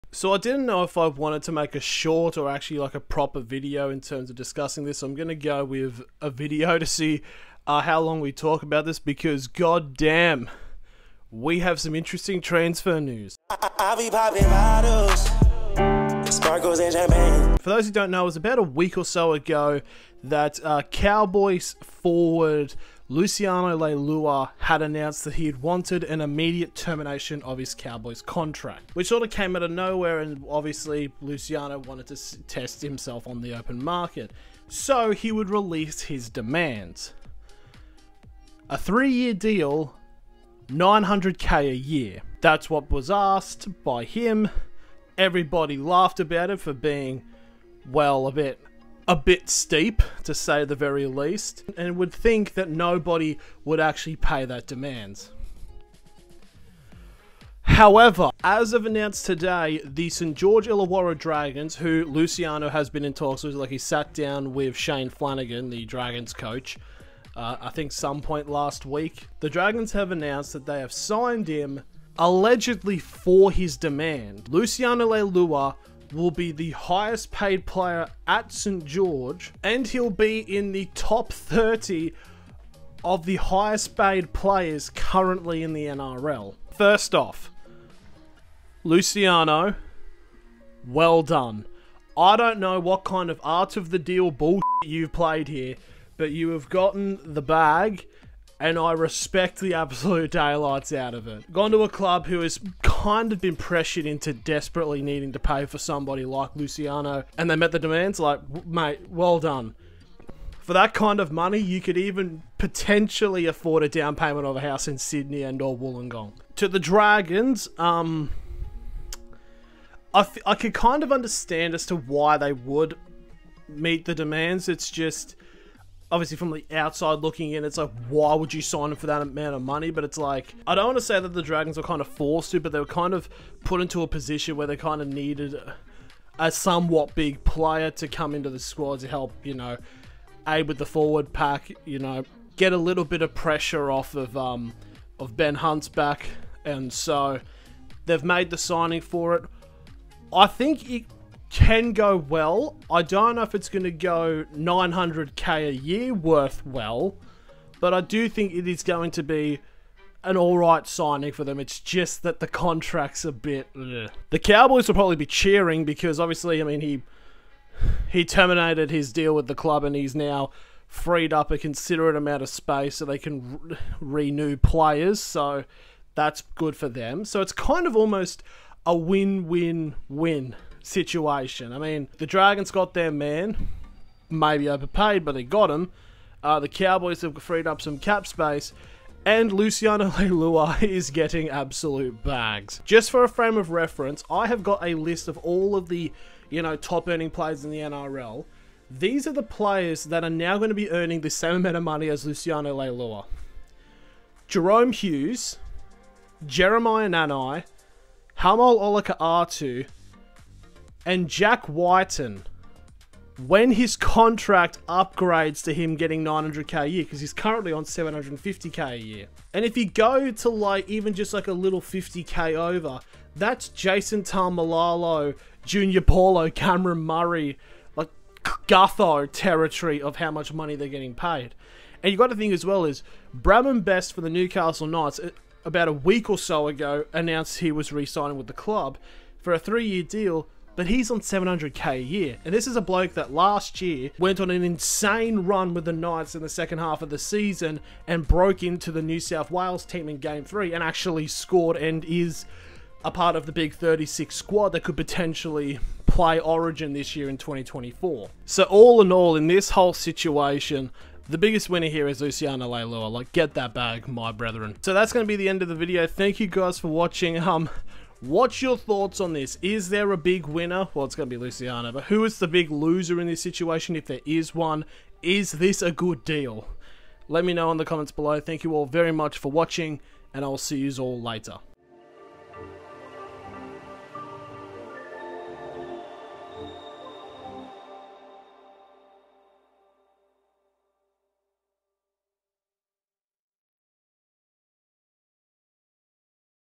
Luciano Leilua Signs $2.7 MILLION Deal With St George Illawarra Dragons
Luciano Leilua terminated his contract with the Cowboys and wanted a deal worth $2.7 Million Deal. And the St George Illawarra Dragons bought him. Here we discuss if the transfer was good.